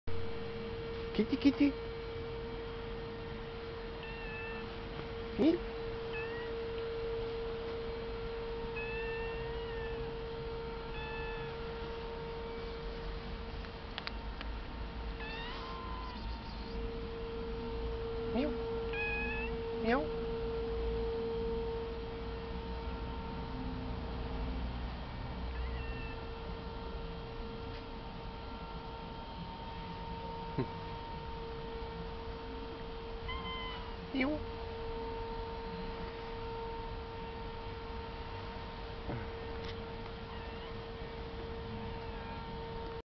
The Cat and The Dark Templar
A lonely cat under a painting of a Dark templar from SC 1.